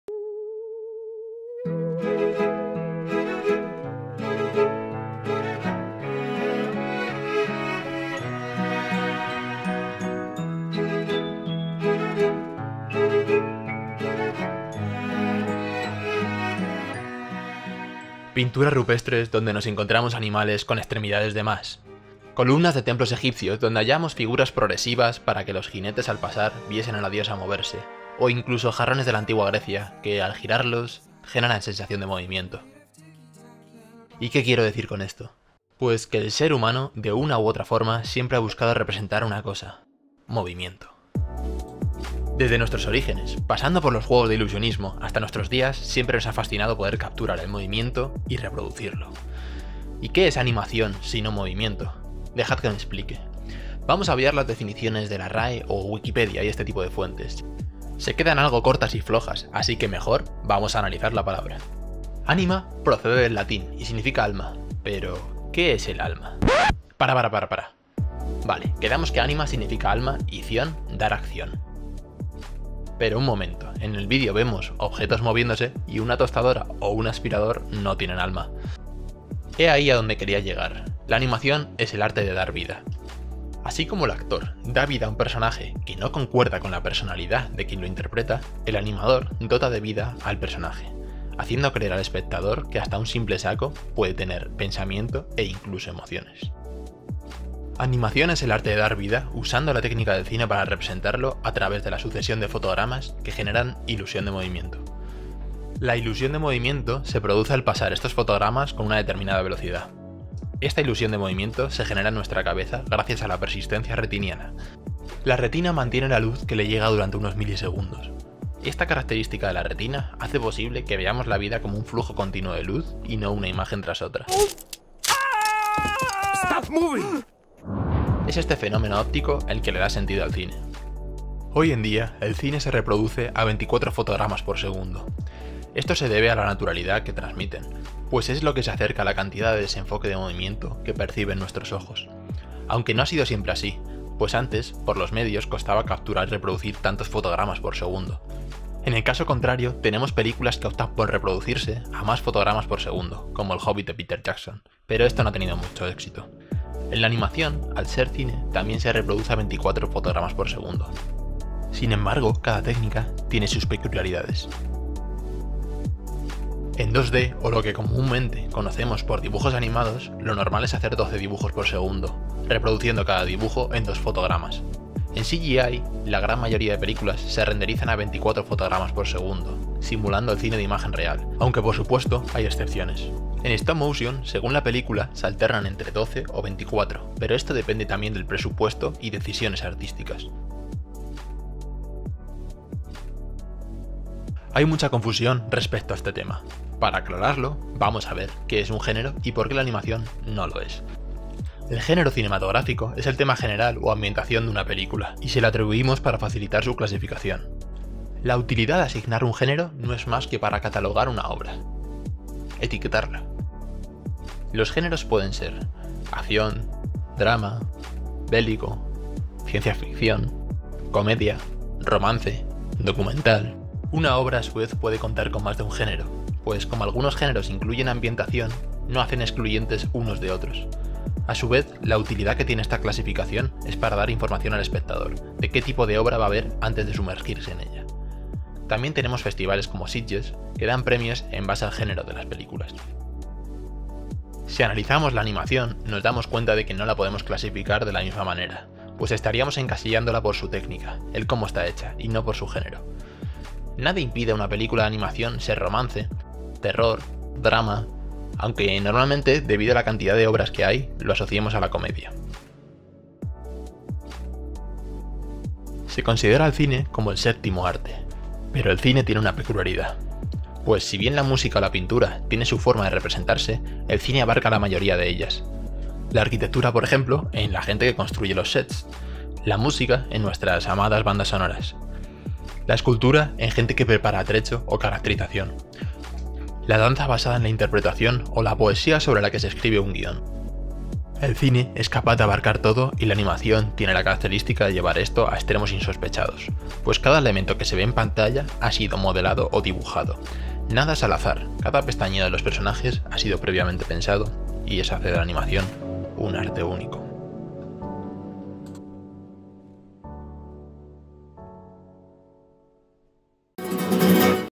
¿Qué es la ANIMACIÓN? 🎥
Una explicación del cine de animación.

 ¿Por qué existe la animación? ¿Por qué nuestros ojos pueden percibirla como real? ¿Es la animación un género?

00:00 Cabecera
00:18 Introducción
00:39 Movimiento
01:54 La persistencia retiniana
03:11 La animación no es género
04:23 El arte de las artes

Todas esas preguntas se responden en este video.